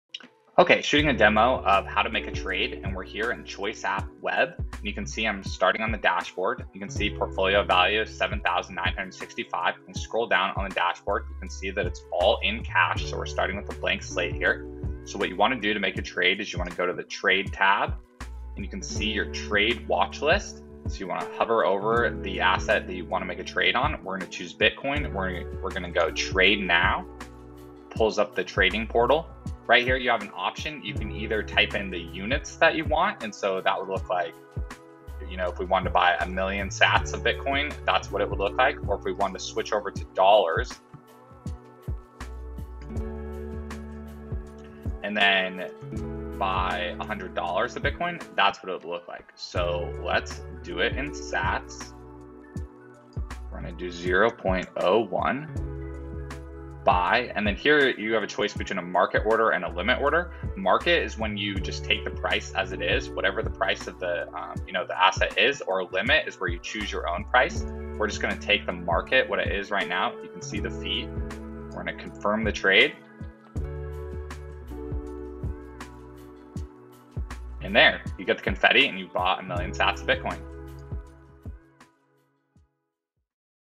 Choice Web App | How to Trade
How to trade bitcoin using Choice web app.